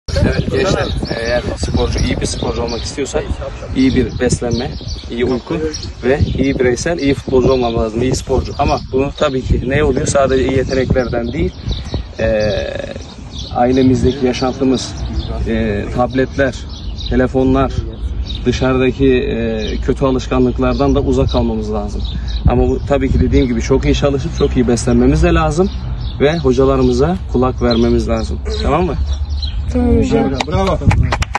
Ünlü futbolcu minik futbolculara önerilerde bulundu
"HRZ Spor Danışmanı" çalışma alanını ziyaret eden ünlü futbolcu Serdar Deliktaş, minik futbolculara önerilerde bulundu.